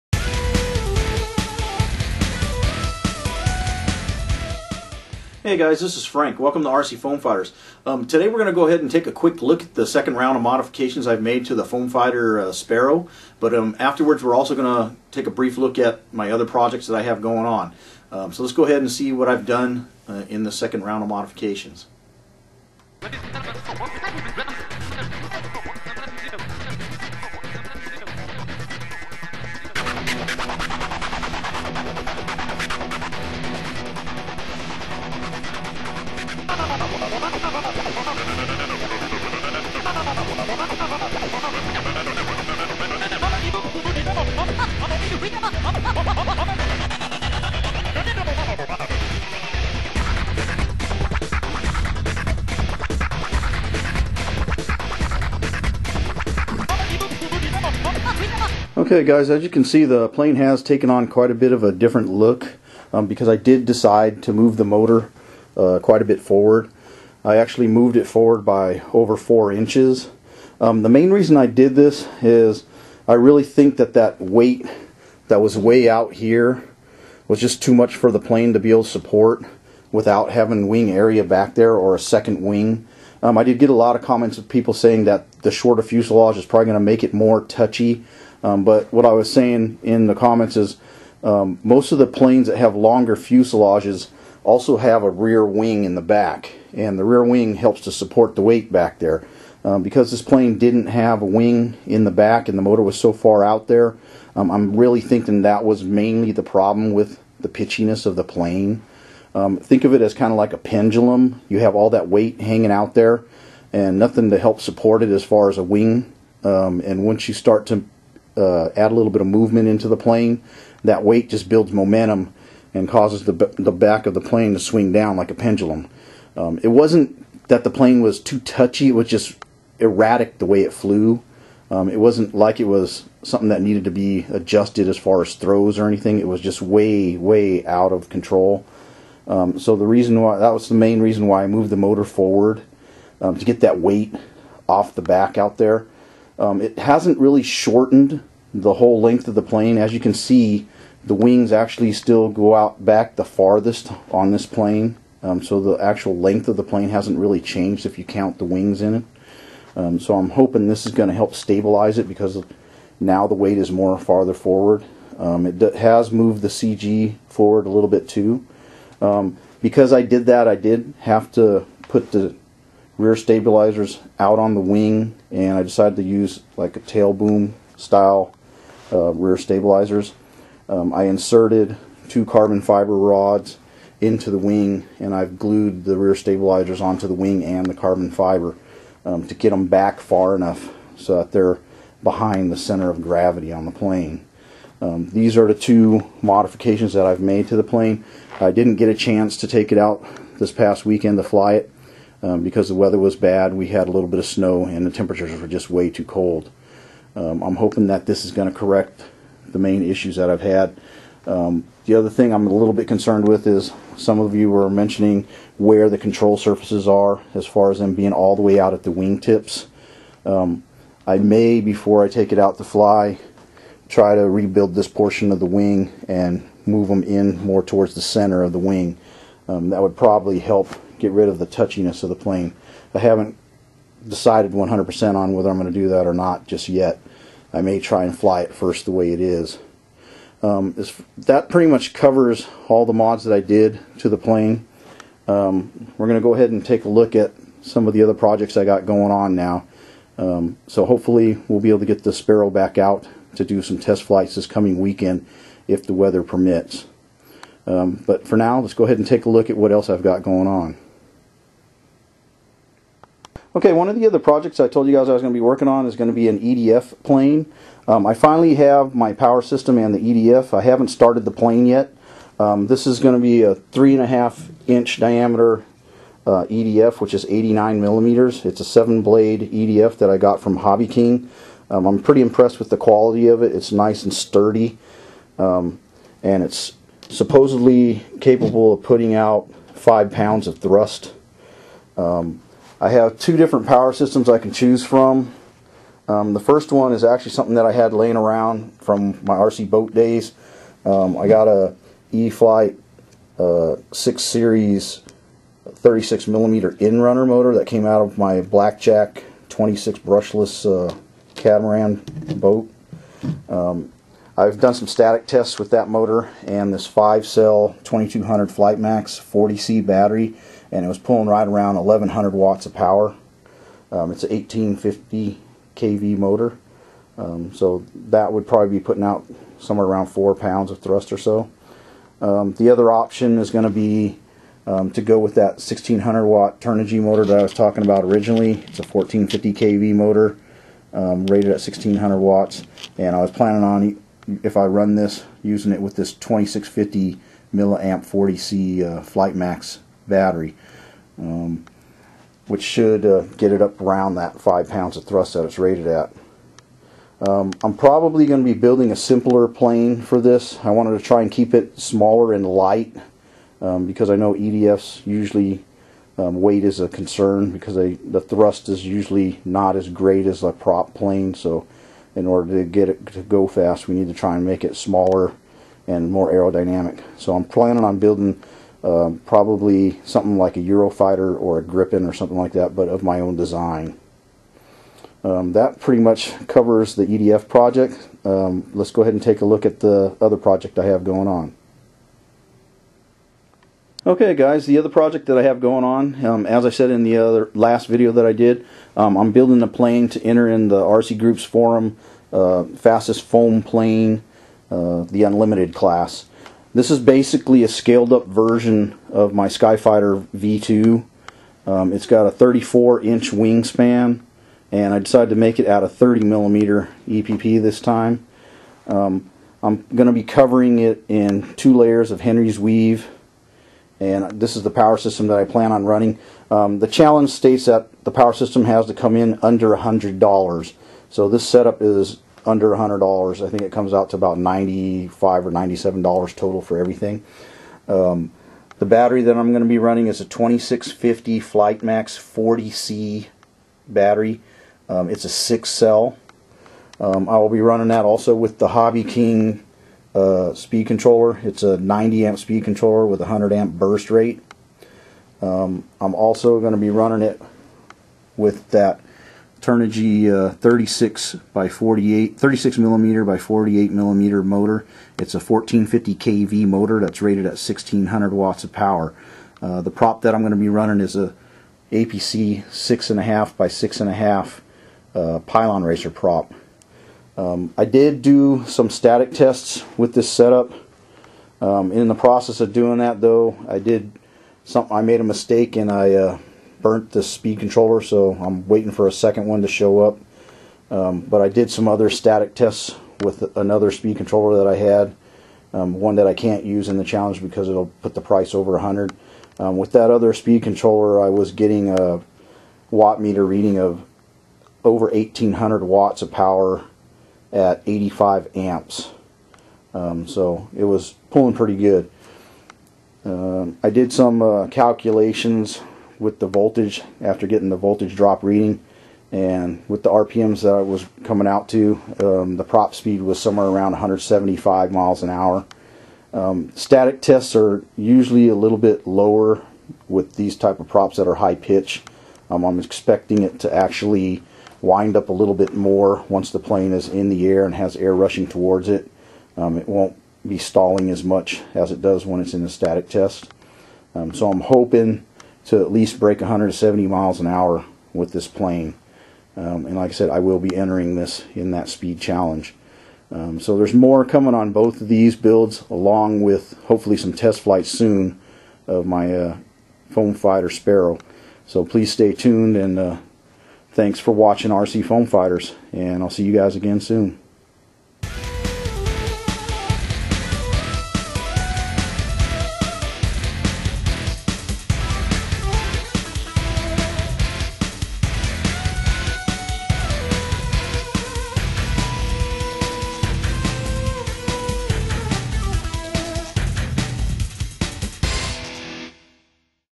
FF-Sparrow additional mods and upcoming projects
FF-Sparrow additional mods and upcoming projects Includes:

Intro

Brief look at additional mods to FF-Sparrow

Breakdown of Mods

Quick look at upcoming projects

conclusion